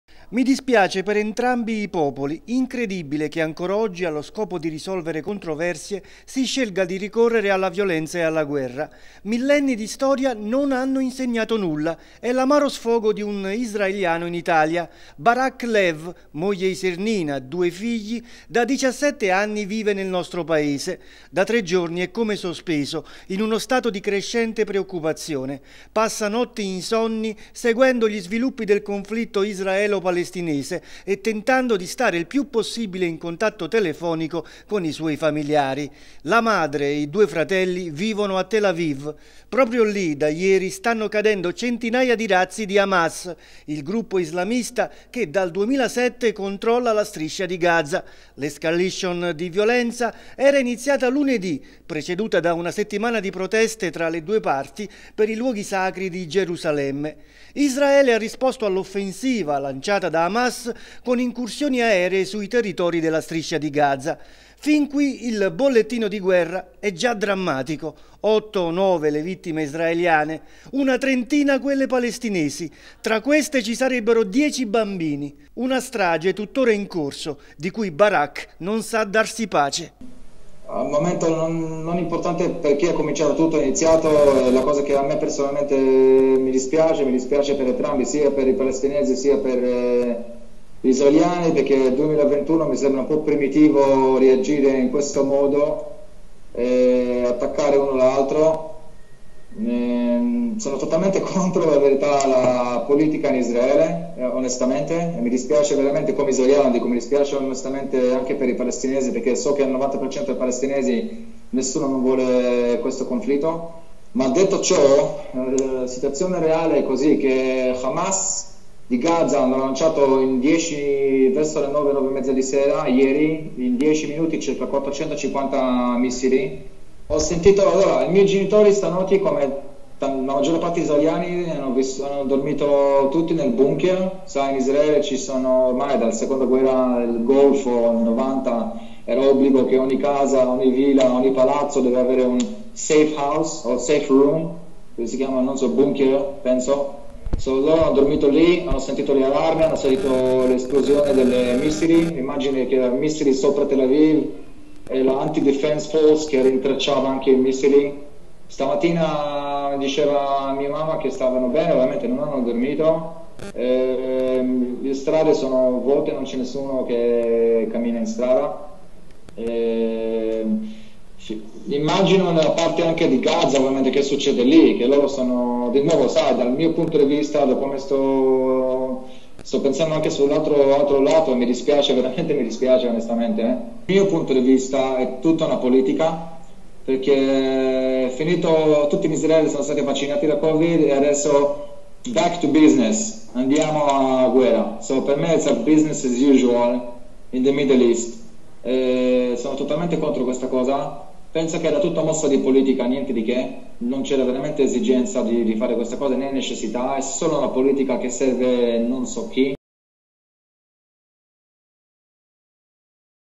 “Mi dispiace per entrambi i Popoli!”
Da un Israeliano residente in Molise parole toccanti circa il conflitto israelo-palestinese che da tre giorni sta infiammando il Medio Oriente…